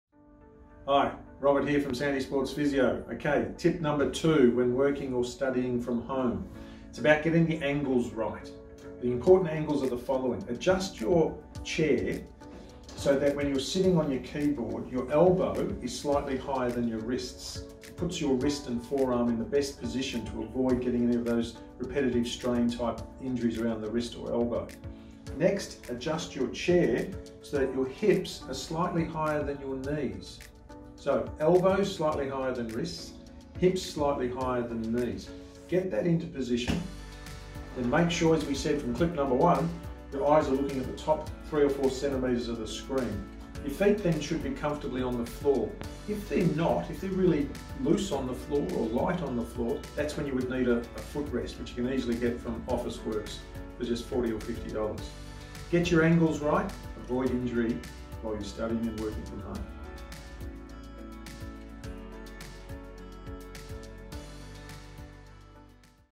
Ergonomics Tips - Ideal arm and leg angles while working at a desk
It's all about angles! Let's take a closer look at the ideal positioning of your arms and legs while working or studying at your desk. This simple tip should help in avoiding long-term injuries like RSI in the elbows and wrists.